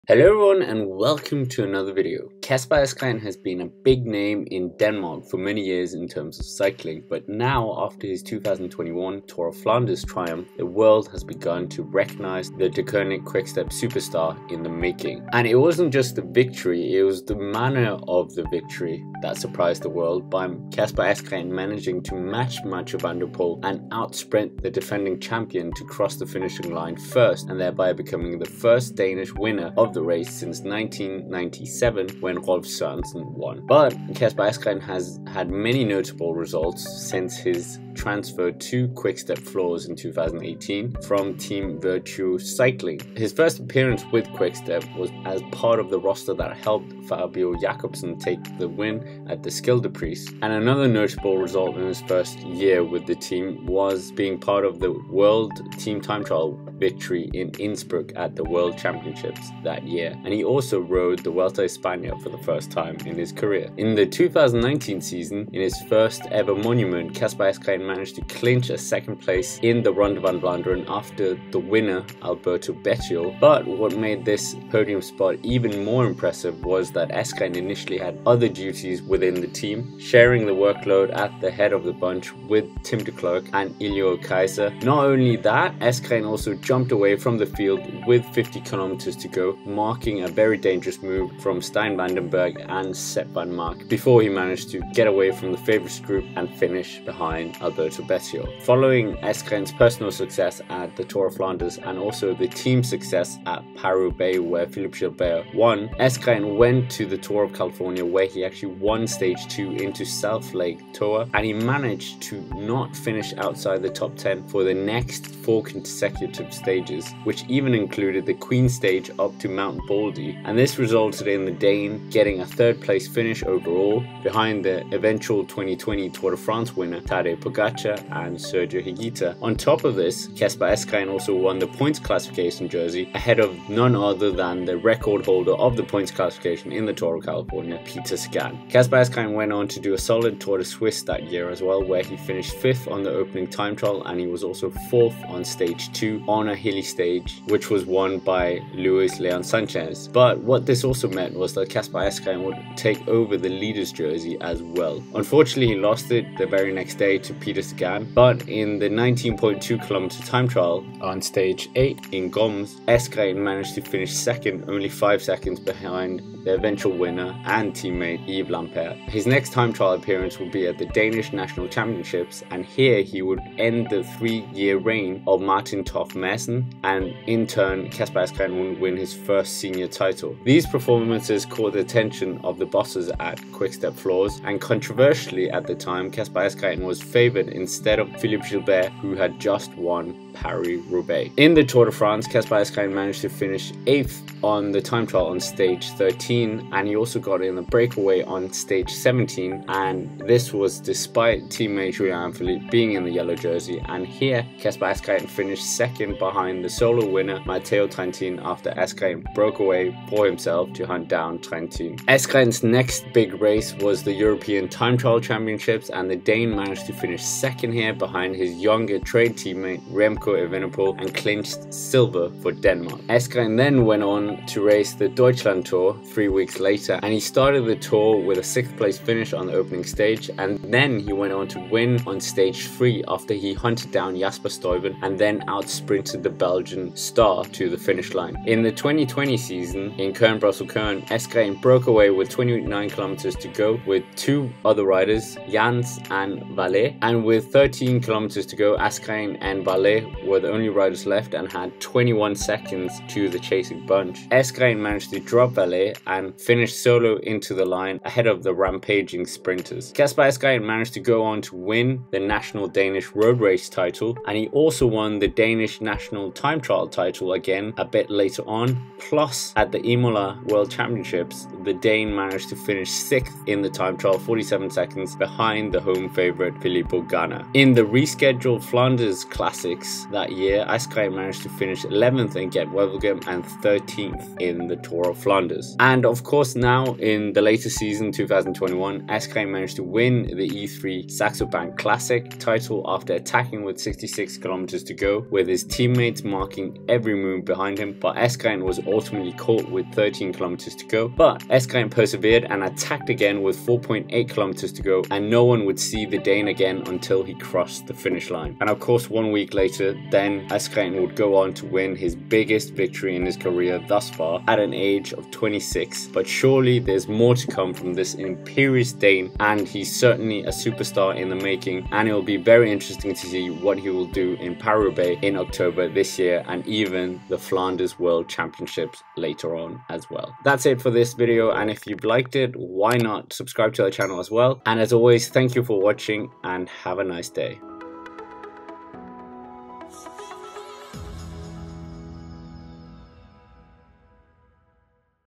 Who Is Kasper Asgreen? The NEXT Superstar of Cycling?
Kasper Asgreen shocked the world when he beat Mathieu at the 2021 Tour of Flanders and he has already achieved a lot of results since he joined Deceuninck–Quick-Step in 2018.